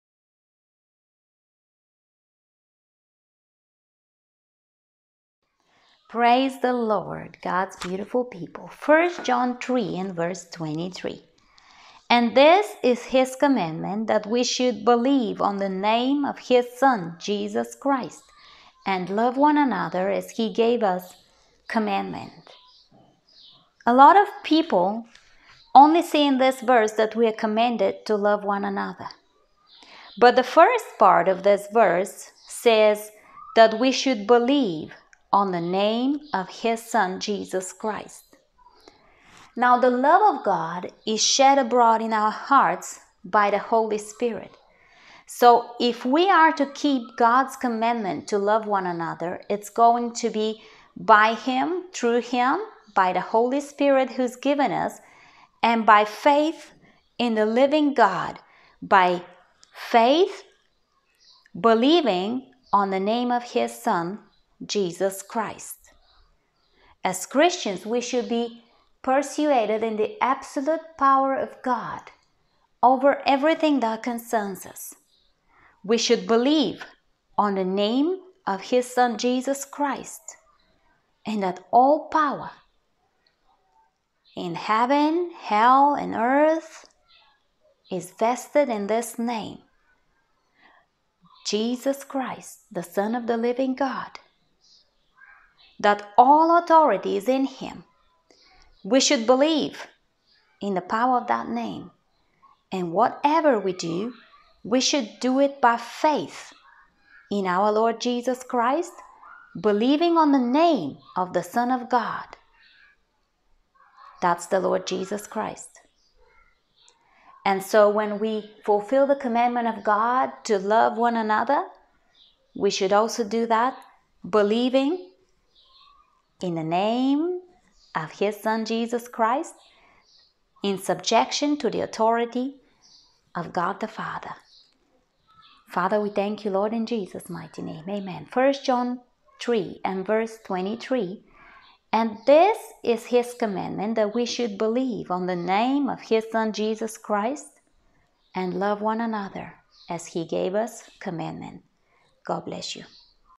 Daily Scripture Day #90
1 John 3:23:

"And this is his commandment, That we should believe on the name of his Son Jesus Christ, and love one another, as he gave us commandment."

The Love of Christ International

Tictoc